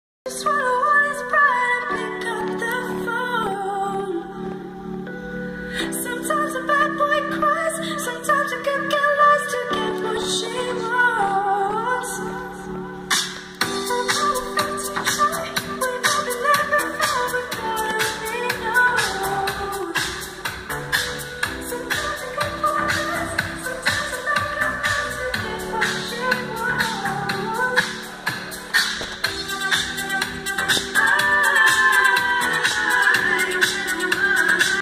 Componentes Coustic Mtx en acción.
Les compartimos la prueba de sonido de este Gol Trend que instalamos:
- Potencia Mtx 75.4
- Caja armada Mtx Jh 4512
- Componentes delanteros Coustic Mtx
- Cables 4 gauge
Todo con el estéreo original, al cual reemplazaremos en segunda etapa.